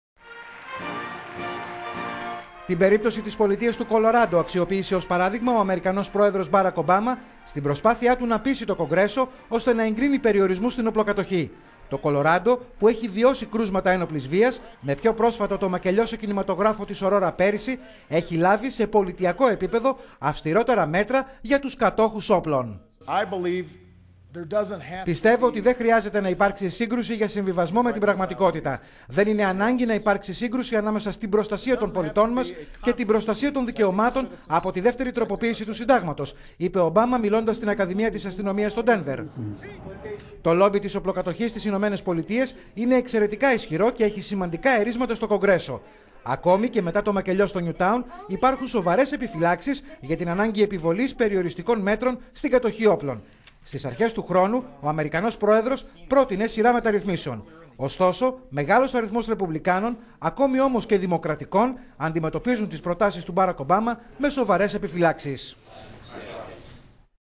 Πιέσεις Ομπάμα στο Κονγκρέσο για το ζήτημα της...
Την περίπτωση της πολιτείας του Κολοράντο αξιοποίησε ως παράδειγμα ο αμερικανός πρόεδρος, Μπάρακ Ομπάμα, στην προσπάθειά του να πείσει το Κονγκρέσο, ώστε να εγκρίνει περιορισμούς στην οπλοκατοχή.

Το Κολοράντο, που έχει βιώσει κρούσματα ένοπλης βίας, με πιο πρόσφατο το μακελειό σε κινηματογράφο της Ορόρα πέρυσι, έχει λάβει σε πολιτειακό επίπεδο αυστηρότερα μέτρα για τους κατόχους όπλων.

"Πιστεύω ότι δεν χρειάζεται να υπάρξει σύγκρουση για συμβιβασμό με την πραγματικότητα. Δεν είναι ανάγκη να υπάρξει σύγκρουση ανάμεσα στην προστασία των πολιτών μας και την προστασία των δικαιωμάτων από την Δεύτερη Τροποίηση του Συντάγματος", είπε ο Ομπάμα, μιλώντας στην Ακαδημία της αστυνομίας στο Ντένβερ.

Το λόμπι της οπλοκατοχής στις Ηνωμένες Πολιτείες είναι εξαιρετικά ισχυρό και έχει σημαντικά ερείσματα στο Κονγκρέσο.

Ακόμη και μετά το μακελειό στο Νιουτάουν, υπάρχουν σοβαρές επιφυλάξεις για την ανάγκη επιβολής περιοριστικών μέτρων στην κατοχή όπλων.

Στις αρχές του χρόνου ο αμερικανός πρόεδρος πρότεινε σειρά μεταρρυθμίσεων.

Ωστόσο μεγάλος αριθμός Ρεπουμπλικάνων, ακόμη όμως και Δημοκρατικών αντιμετωπίζουν τις προτάσεις με επιφύλαξη.